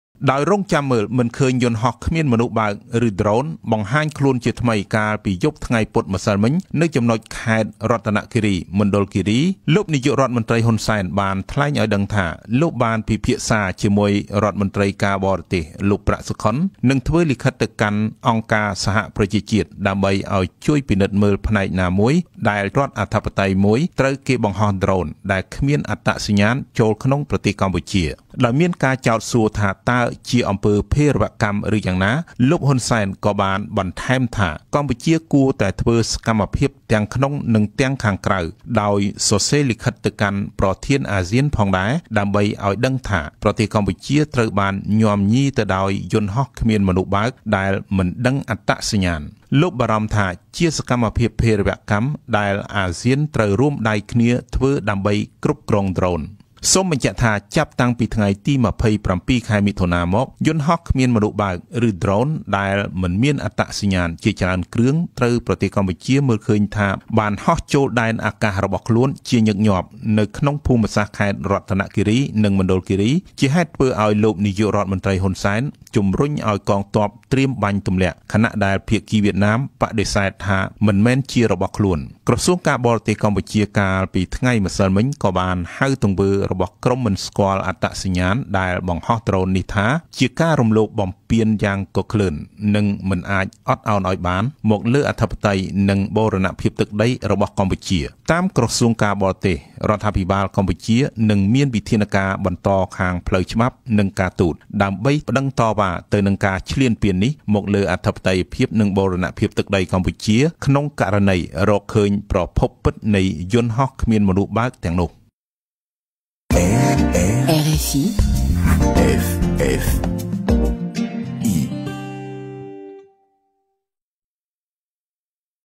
លោក ហ៊ុន សែន គ្រោងប្តឹងទៅអង្គការសហប្រជាជាតិនិងអាស៊ាន រឿងដ្រូនអនាមិកហោះចូលដីខ្មែរ
លោកនាយករដ្ឋមន្ត្រី ហ៊ុន សែន បានថ្លែងថាលោកបានពិភាក្សាជាមួយរដ្ឋមន្ត្រីការបរទេសហើយថា នឹងធ្វើលិខិតទៅកាន់អង្គការសហប្រជាជាតិប្រសិនបើដ្រូនមិនស្គាល់អត្តសញ្ញាណបន្តហោះចូលទឹកដីកម្ពុជាទៀត។ ដំណឹងនេះក៏ត្រូវជូនទៅប្រធានអាស៊ានដែរ ដើម្បីឲ្យអាស៊ានចូលរួមដោះស្រាយ។ ការថ្លែងរបស់លោកនាយករដ្ឋមន្ត្រីដូច្នេះ គឺបានធ្វើឡើងនៅក្នុងឱកាសដែលលោកជួបសំណេះសំណាលជាមួយកម្មករ កម្មការិនីជិត២ម៉ឺននាក់ នាព្រឹកថ្ងៃព្រហស្បតិ៍នេះនៅក្នុងខេត្តកណ្តាល។